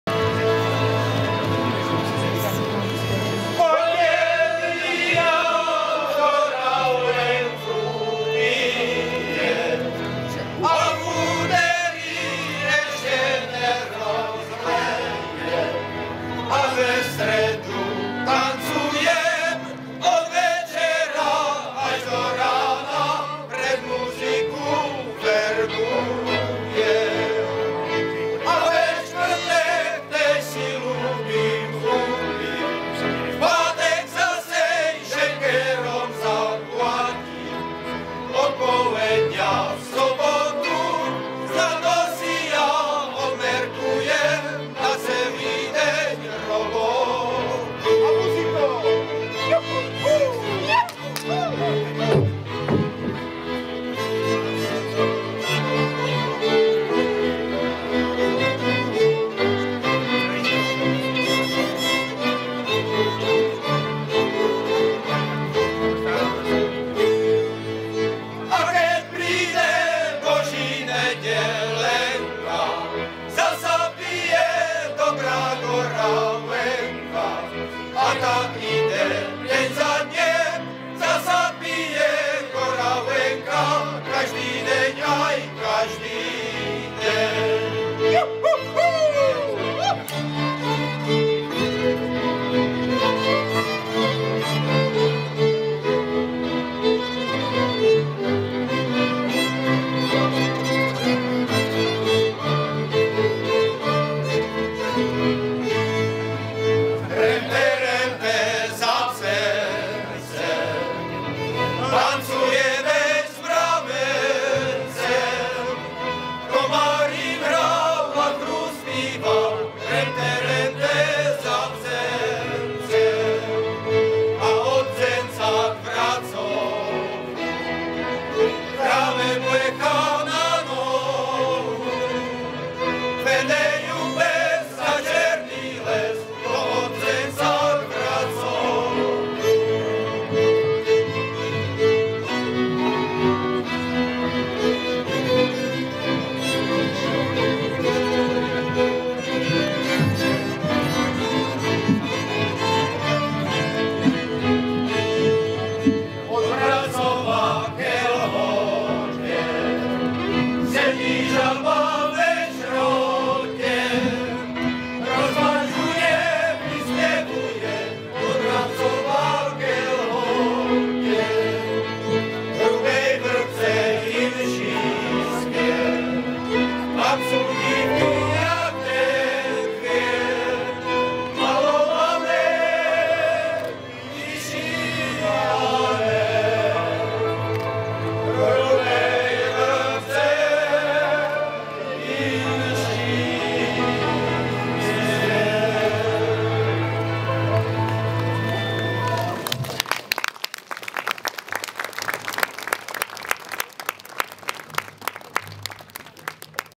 PRÁVĚ DNES - HRUBÁ VRBKA - oslavy 650 let založení horňácké obce 17.
- pěvecký sbor mužů  z Hrubéj Vrbky má výborný kolektiv vedený Jurků Miškeríkem

- hudebně je doprovodiua HCM Martina Hrbáča (72), rodáka z Hrubéj Vrbky